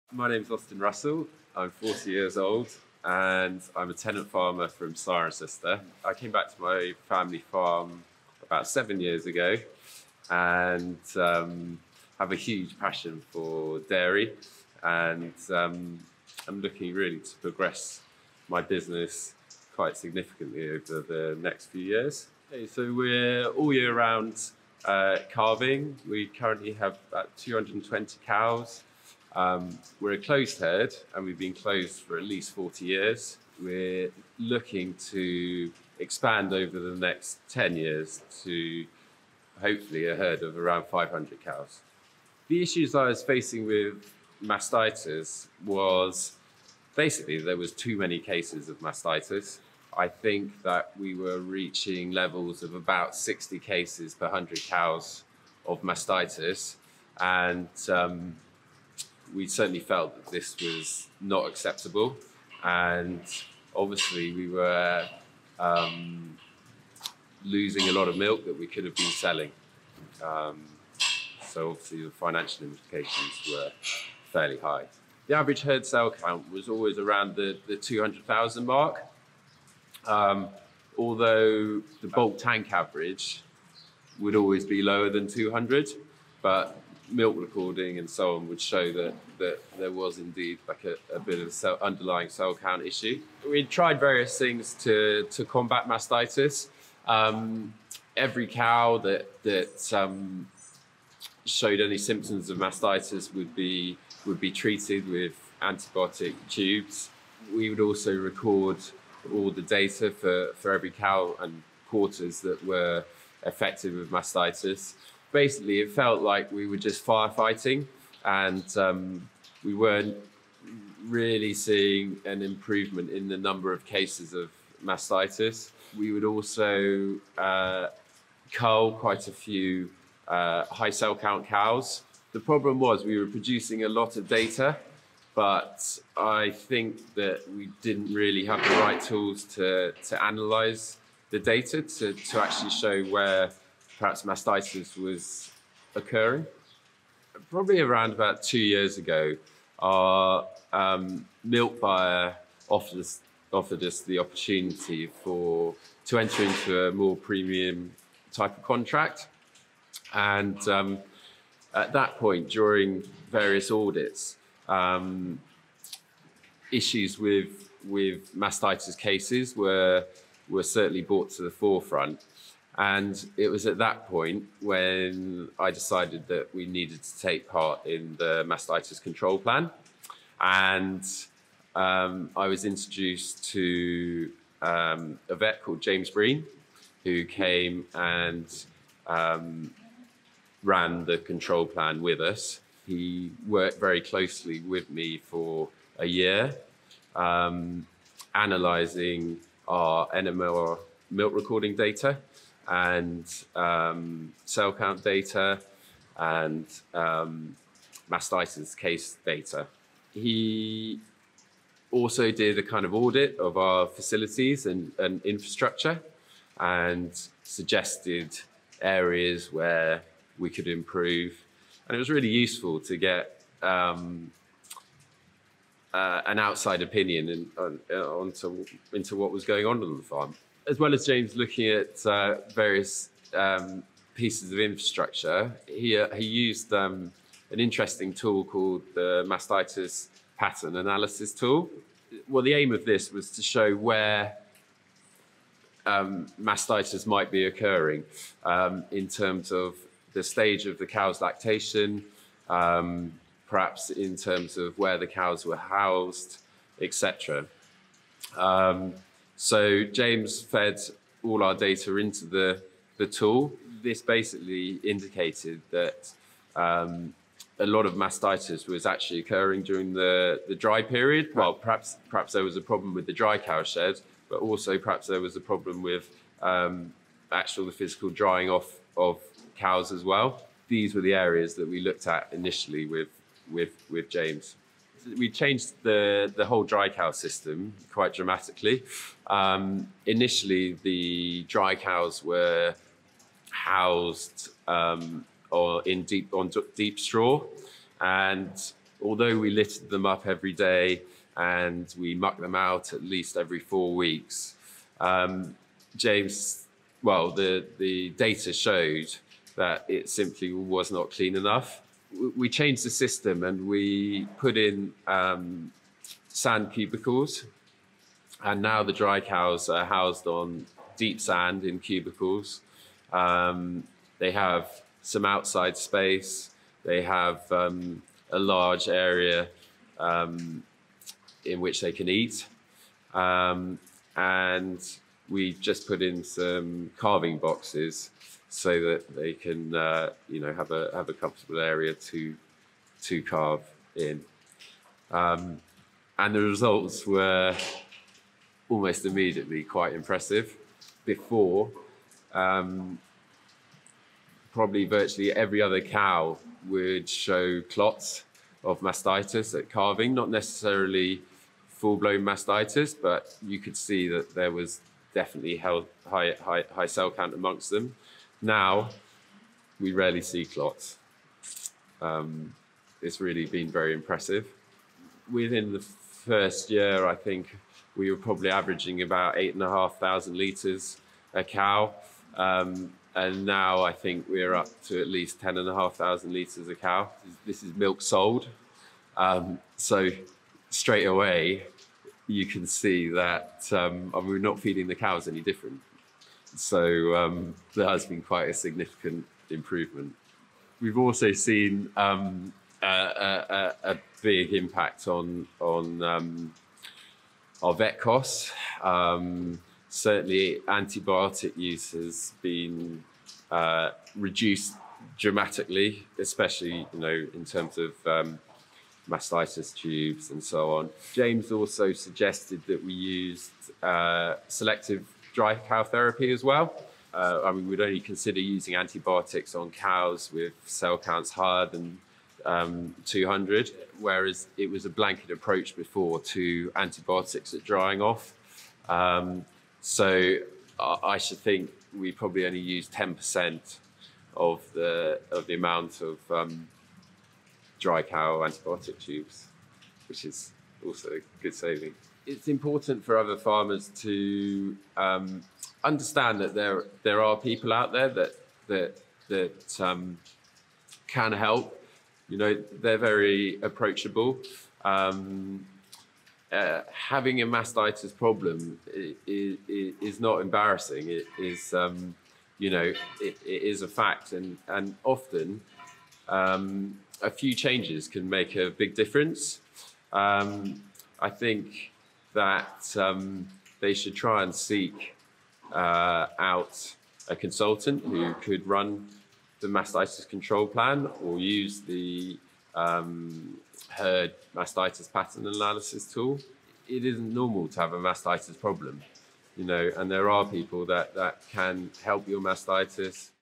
Mastitis turnaround yields growth
Austin Russell discusses how identifying when the majority of mastitis infections occurred on his farm using the Mastitis Pattern Analysis Tool helped him address underlying issues and realise additional benefits.